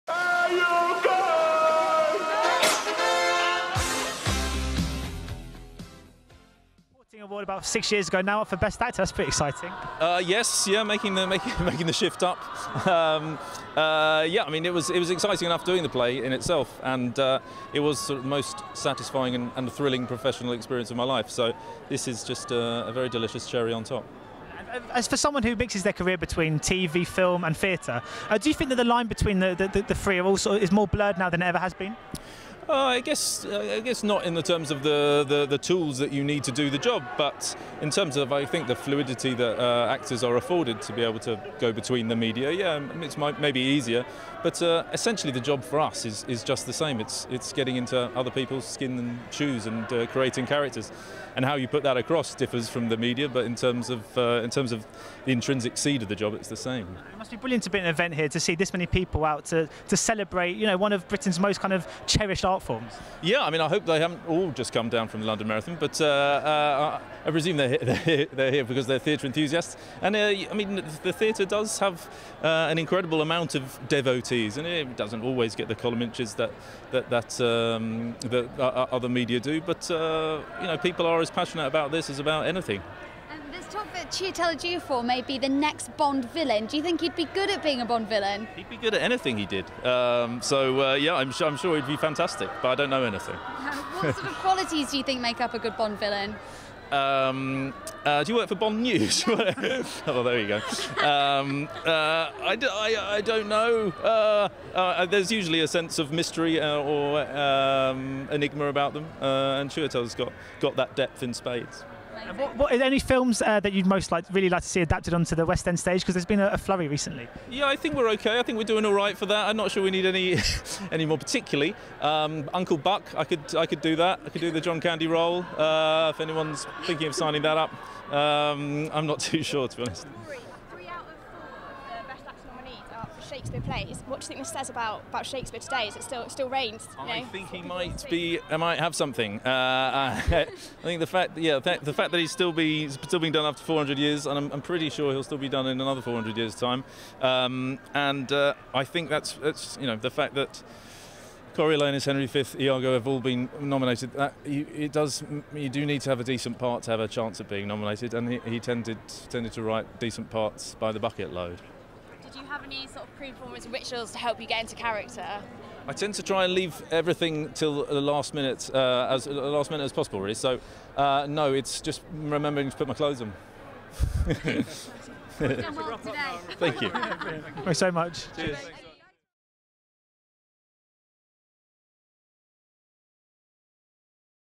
Rory Kinnear Interview - The Olvier Awards 2014
Stefan Pape from HeyUGuys interviews Rory Kinnear at the 2014 Olivier Awards in London.